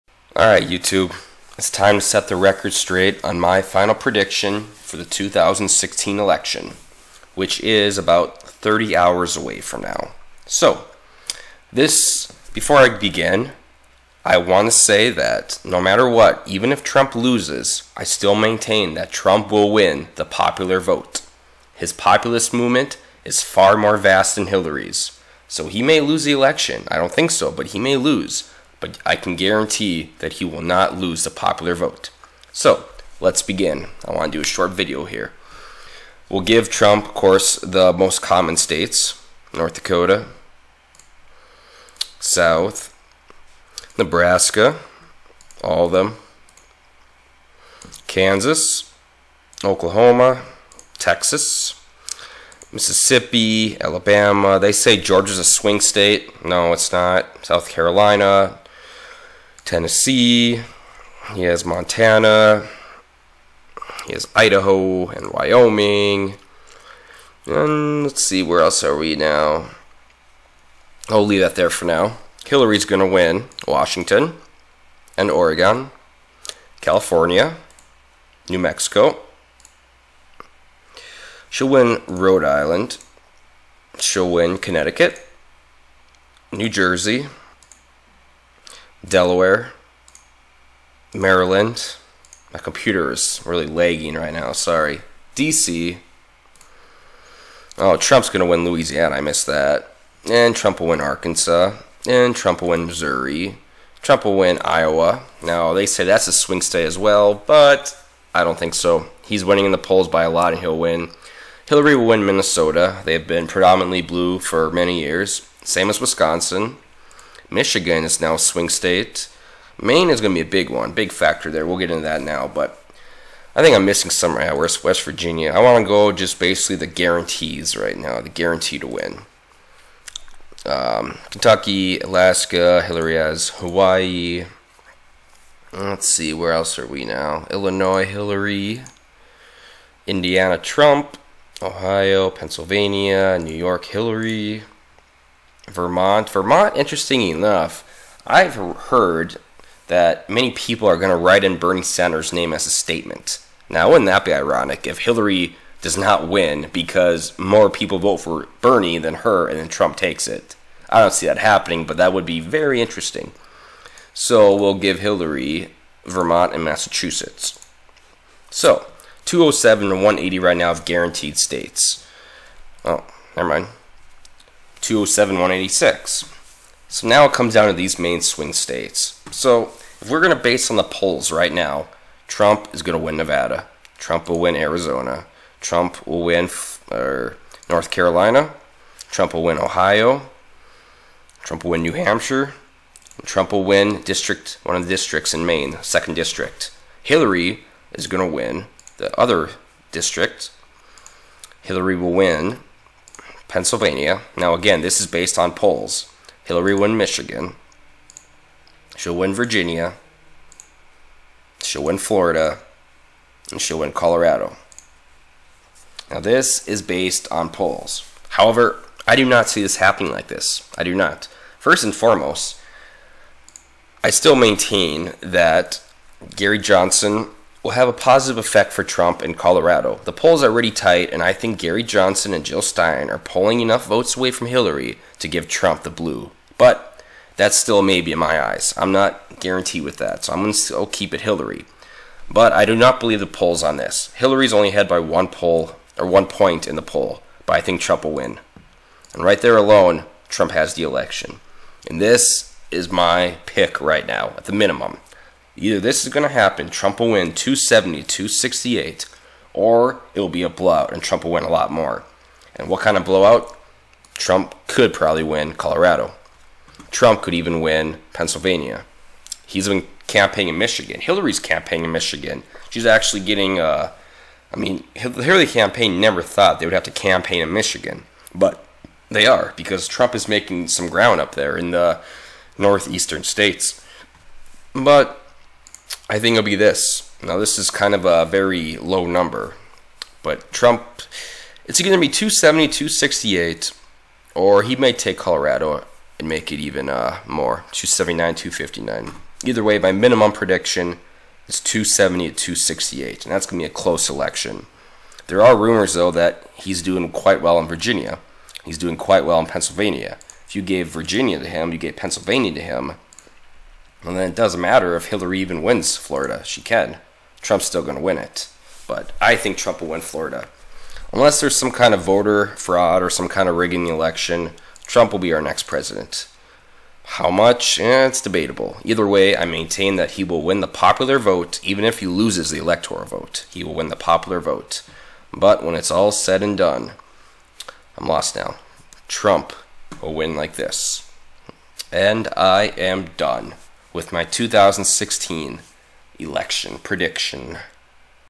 2016 Election Final Prediction - 30 hours From Election Day
Trump will win the popular vote, even if he loses the electoral college.  Although it's possible Hillary could win, I believe it unlikely, and here's how Trump will win.